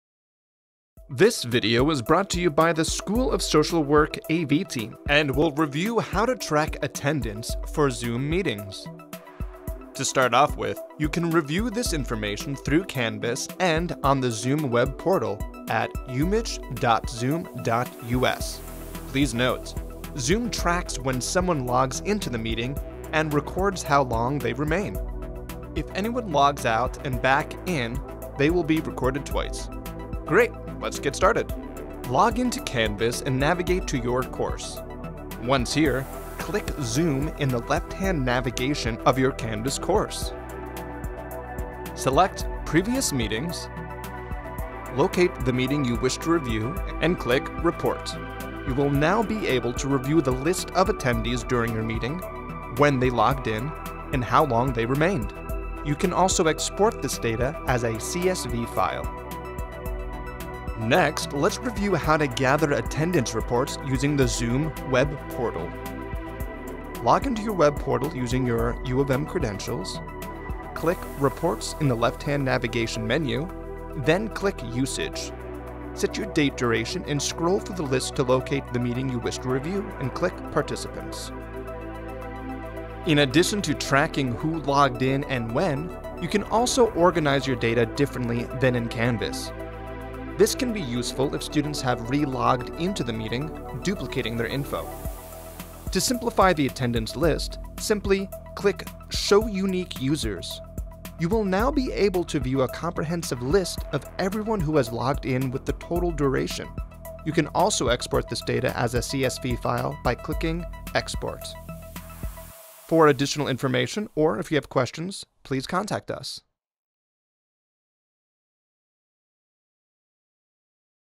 Tracking Attendance for Zoom Meetings
This video reviews the basic functions for the video conferencing application, Zoom along with how it integrates with Canvas.

0:00-0:30 | Intro
0:30-1:01 | Collecting Attendance Data in Canvas
1:01-1:52 | Collecting Attendance Data from the Zoom Web Portal
1:52-2:01 | Contact Information

Video created by Phil M. Boos